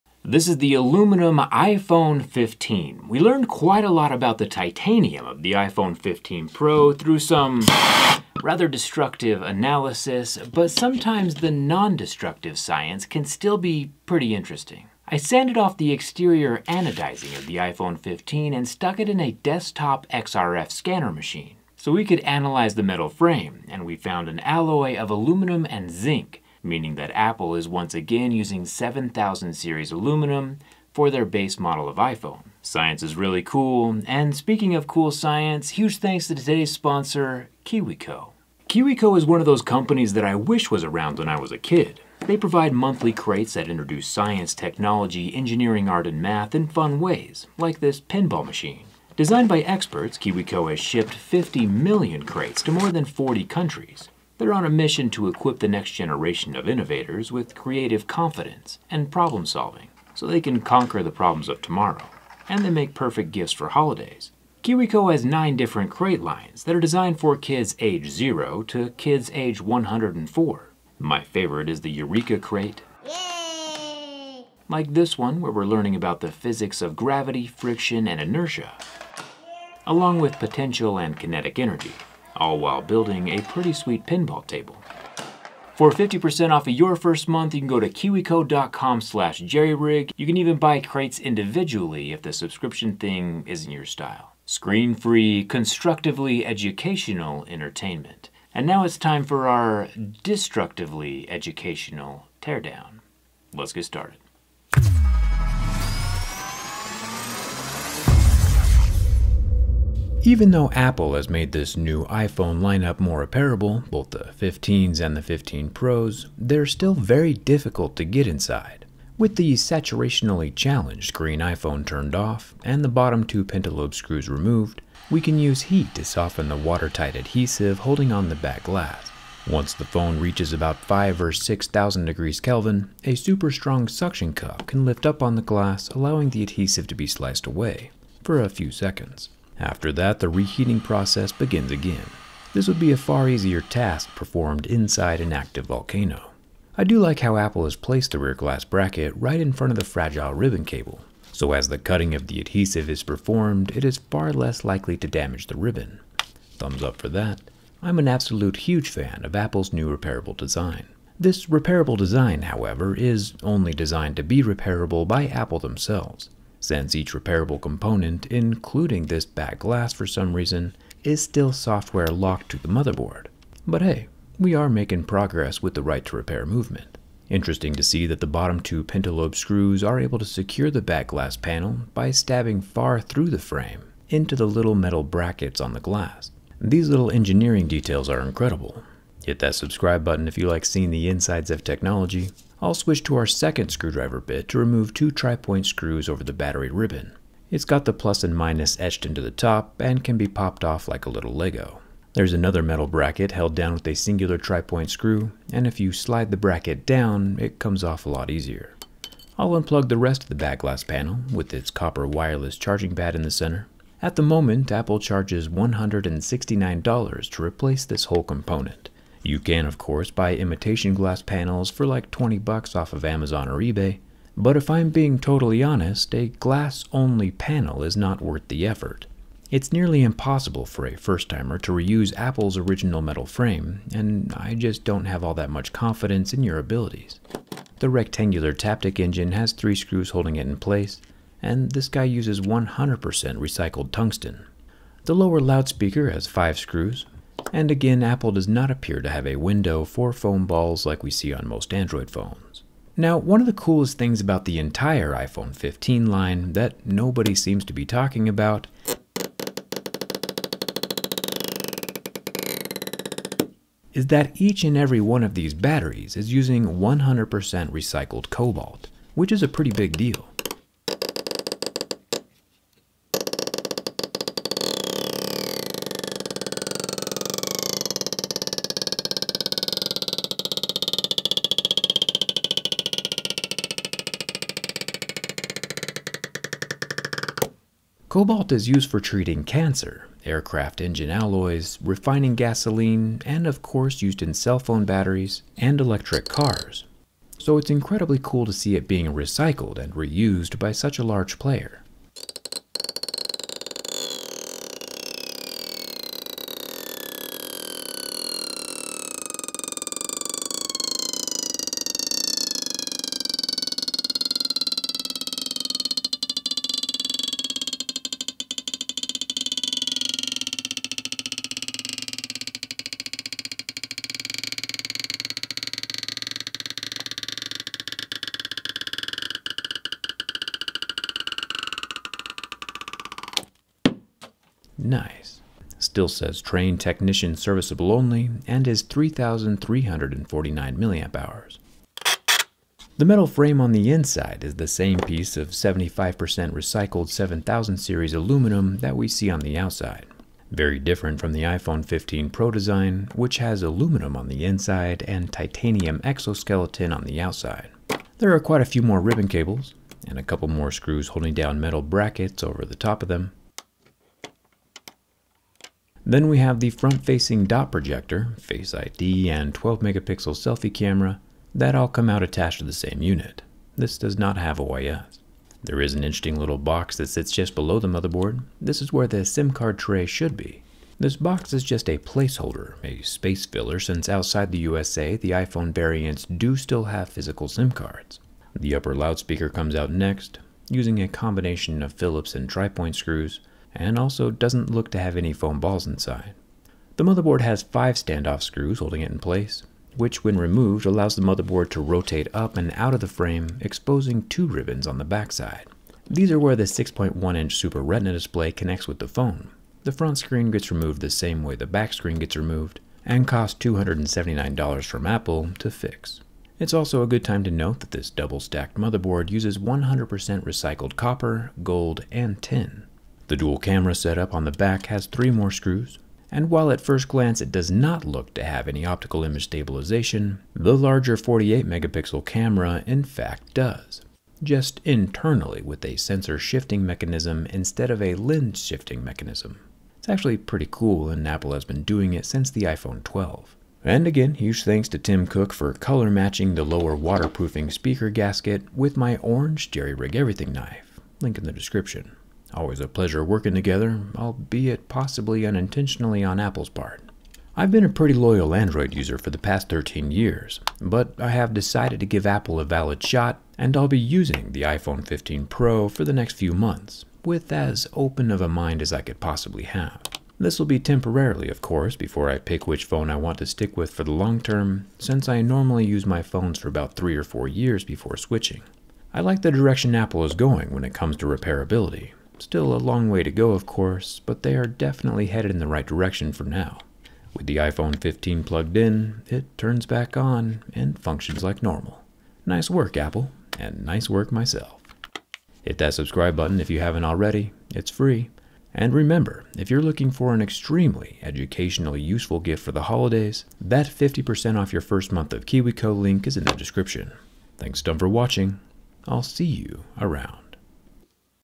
iPhone 15 Teardown - Why is nobody talking about this?!
And being able to reuse the cobalt for ALL of the new iPhone 15 lineup is incredibly awesome. Nice work apple. I would love to see other companies use as many recycled materials as Apple does on their new iPhone 15 and iPhone 15 pro smartphones.

Plastic Pry tools
The camera I used to film this video (GH6
This is the drone I use